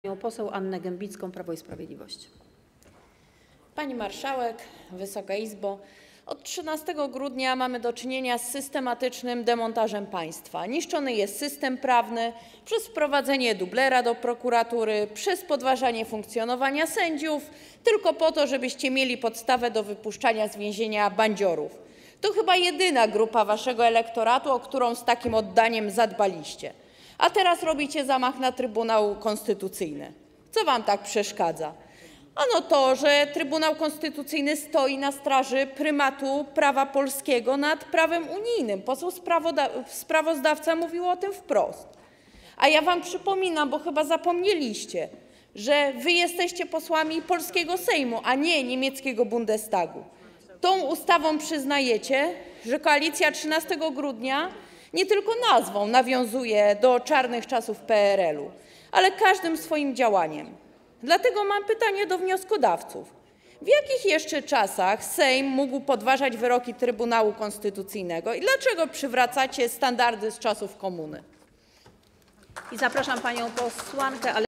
Gembicka: od 13 grudnia mamy do czynienia z systematycznym demontażem państwa!
16. posiedzenie Sejmu - dzień pierwszy. 23 lipca 2024 r.